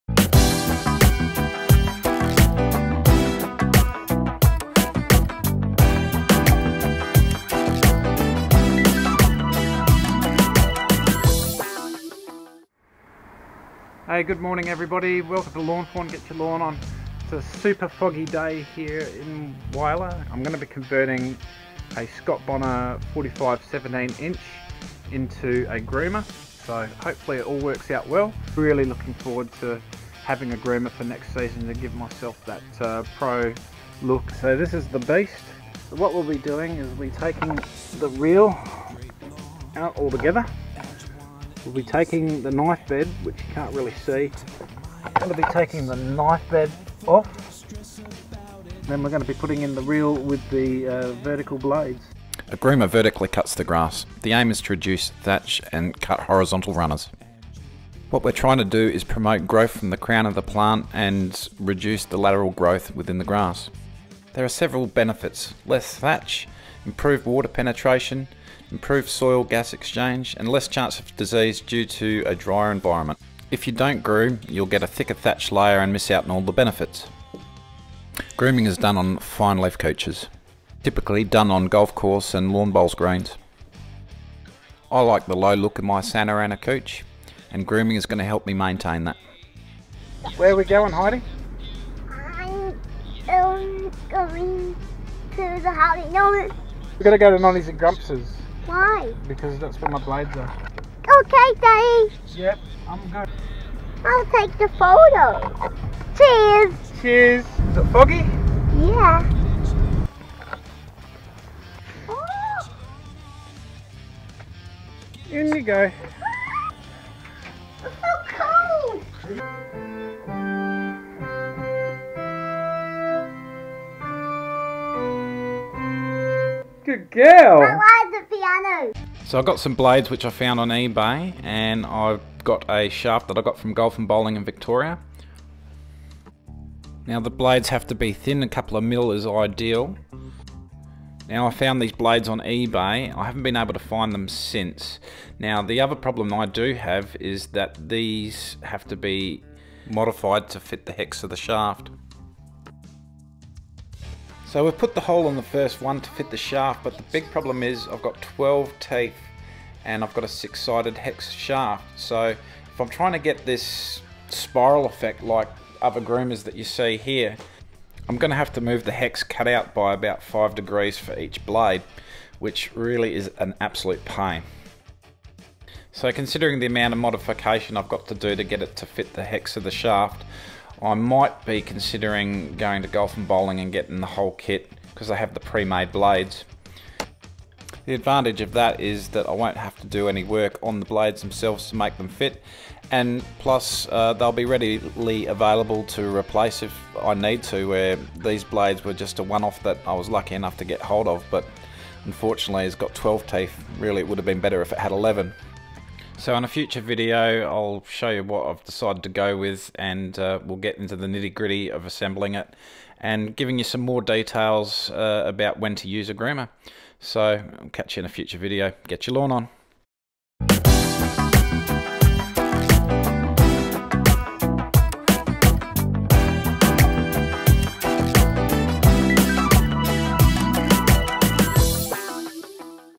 Making a Groomer Part 1
I am converting a Scott Bonnar 45 17" mower to a Groomer to vertically cut the grass.